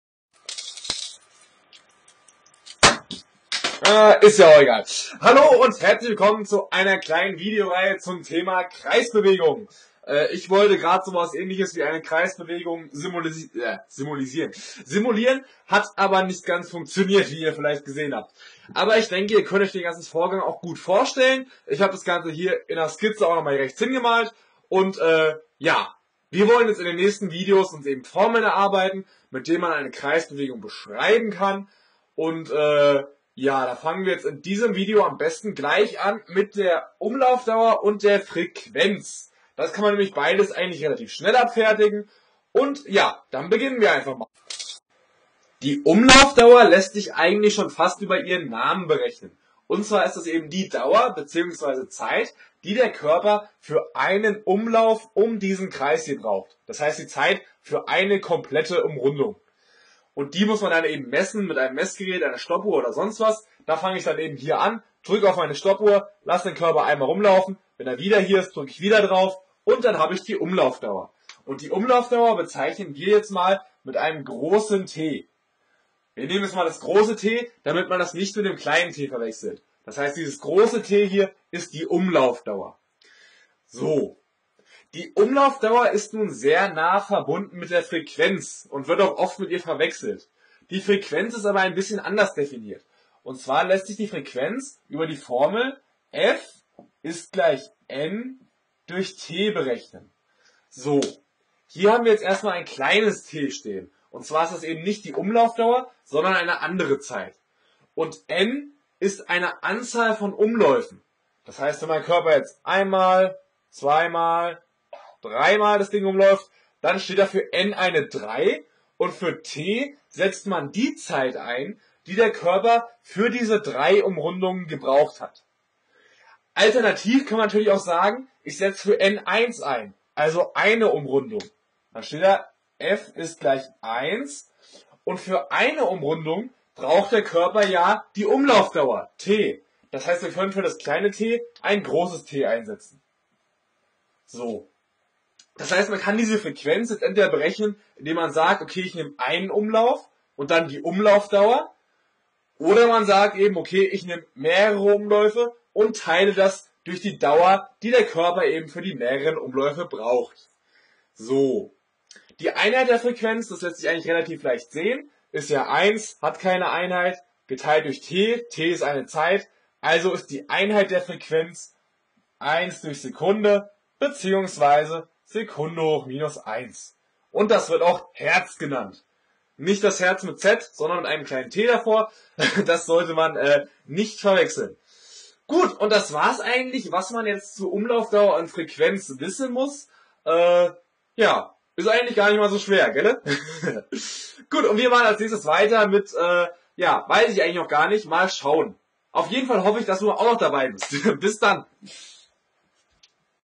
Frequenz und Umlauddauer
Rotation, auch Rotationsbewegung, Drehung, Drehbewegung oder Gyralbewegung, ist ganz allgemein die Bewegung eines Körpers oder eines Bezugssystems um eine gemeinsame Rotationsachse. Der Begriff wird sowohl für eine einmalige Drehung um einen bestimmten Winkel gebraucht als auch für eine fortlaufende Bewegung mit einer bestimmten Winkelgeschwindigkeit. Die Rotationsachse kann, muss aber nicht durch den Massenmittelpunkt des Körpers gehen. Von der hier behandelten Rotation zu unterscheiden ist die Kreisbewegung, bei der ein Körper ohne Änderung seiner Ausrichtung auf einem Kreis umläuft und sich die Punkte des Körpers alle auf gleich großen, gegeneinander versetzten Kreisen bewegen. Nur bei der Bewegung einer Punktmasse fallen die beiden Bewegungsformen zusammen.